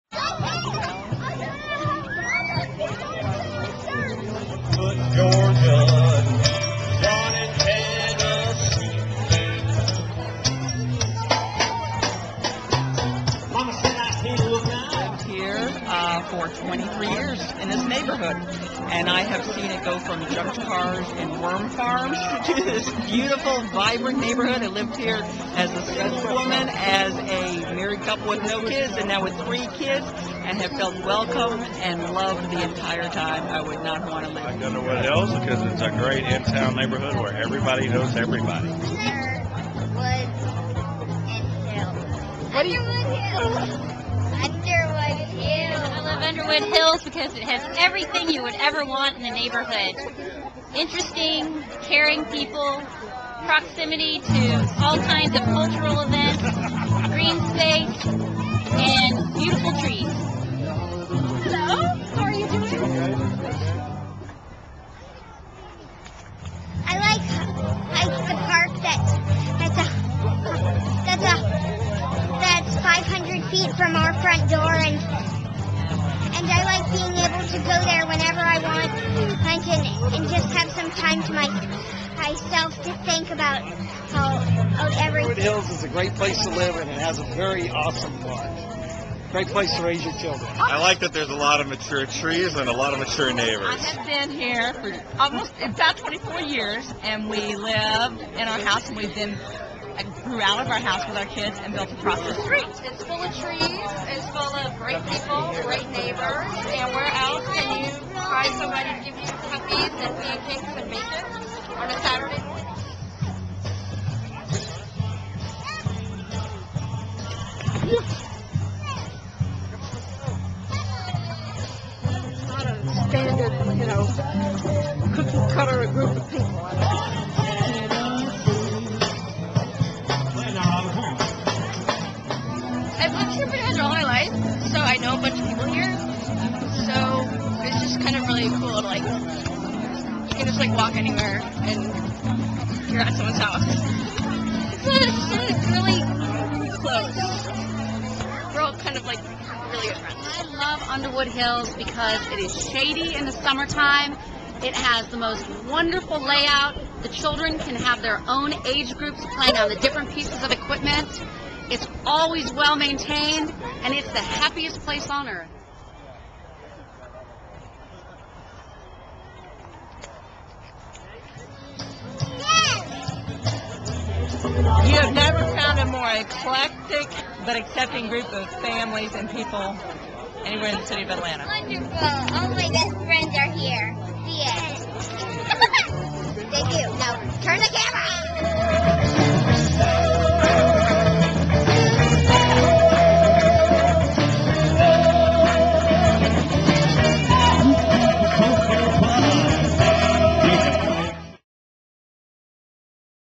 Underwood Hills - A Great Place To Be
Residents of the City of Atlanta neighborhood, Underwood Hills, talk about why they live in Underwood Hills. And why it's a great place to be.